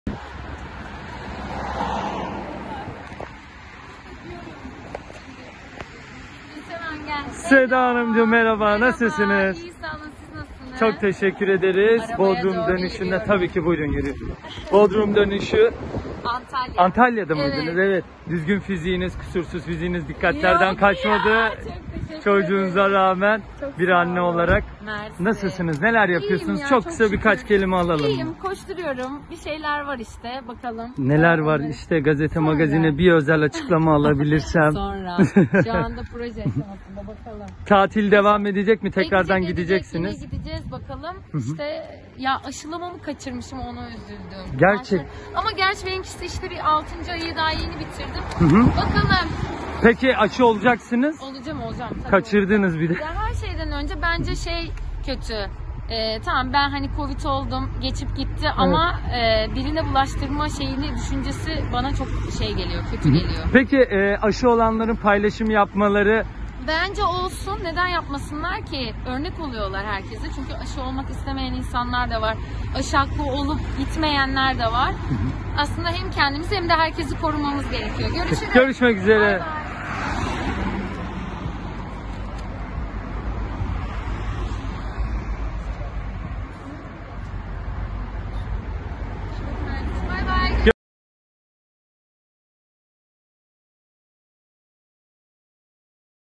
Seda Bakan "Aşı olanlar paylaşım yapsınlar"
Seda Bakan Yeniköy'de görüntülendi. Aşı takvimini kaçırdığını söyleyen ünlü oyuncu, tekrar randevu alıp aşı olacağını söyledi. Bunların dışında yeni projelerinin olacağını belirtti. Aşı olurken fotoğraf paylaşanlarla ilgili olarak da bu hareketin örnek olabileceğini, yapılmasının güzel olduğunu söyledi.

En Taze Magazin Haberleri